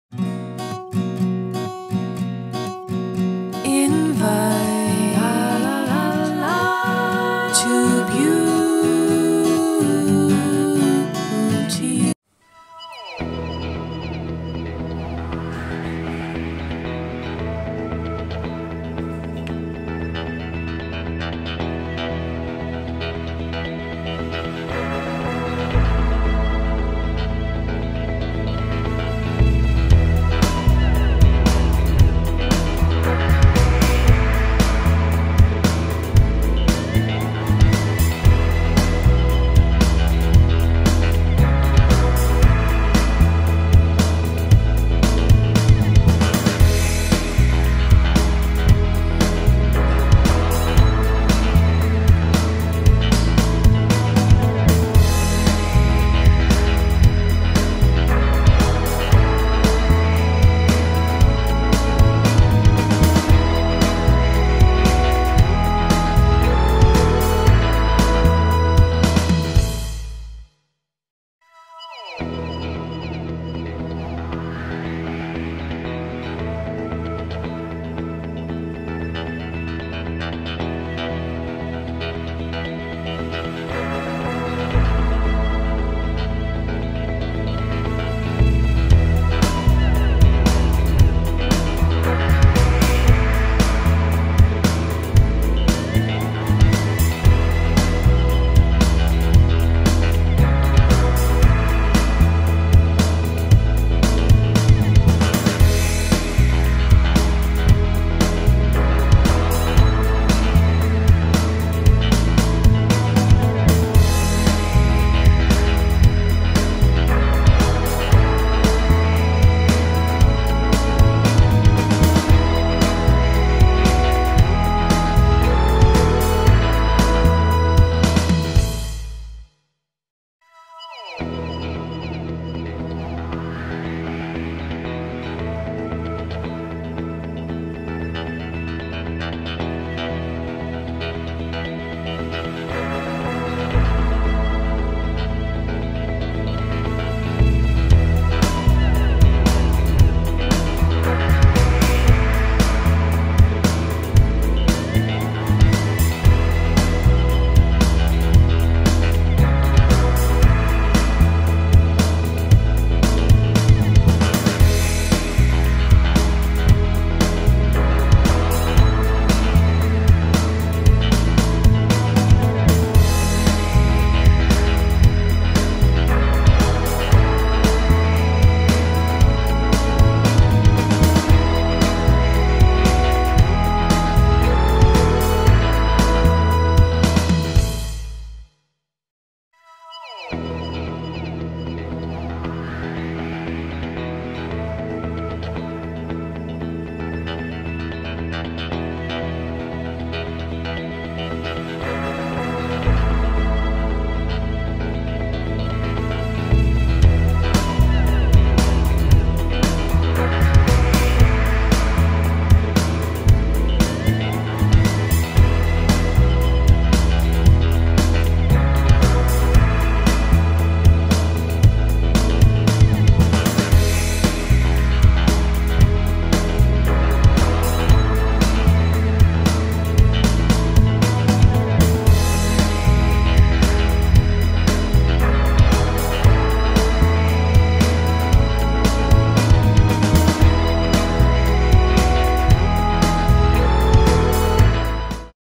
Bedtime routine
In deze video laat ik zien hoe mijn bedtime routine eruit ziet onder genot van een lekker muziekje! Enjoy!

Volg Invitetobeauty.nl op social-media!